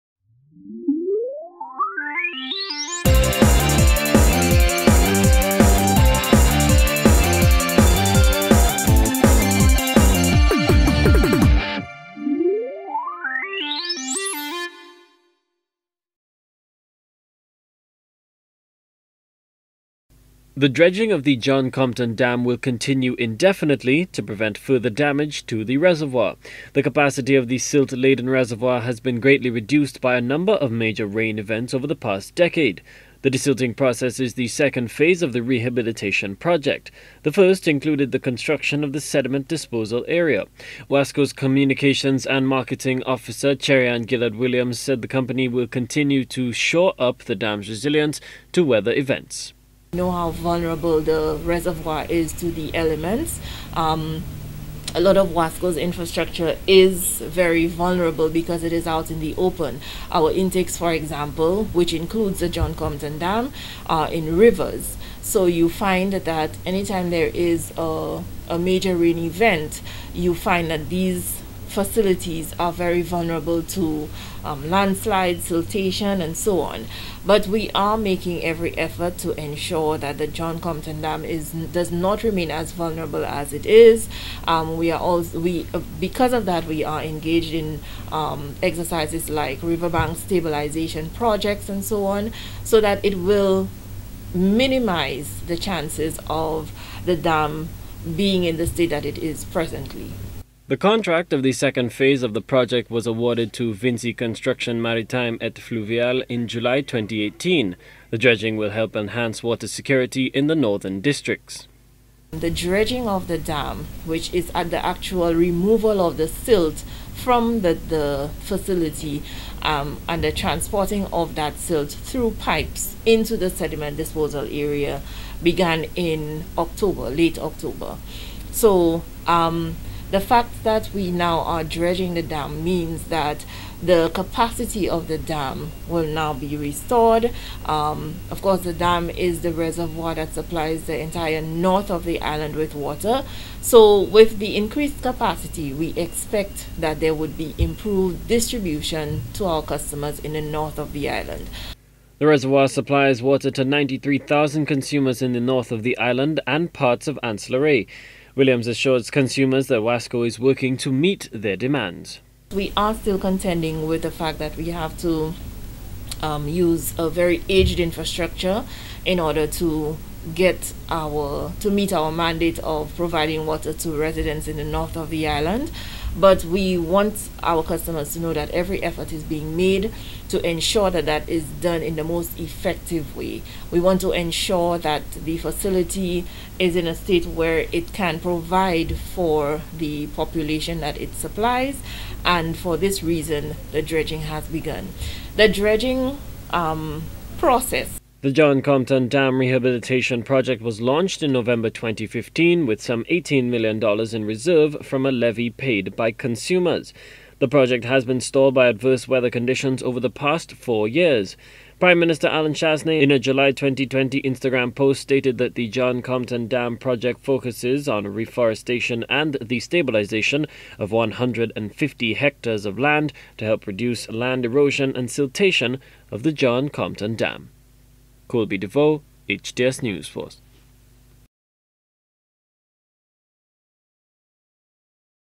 DREDGING WORKS TO CONTINUE AT JOHN COMPTON DAM
The water and sewage company (WASCO) says it is ensuring that the John Compton dam will not be vulnerable to the elements.
Dredging works have finally commenced on the reservoir after much delay.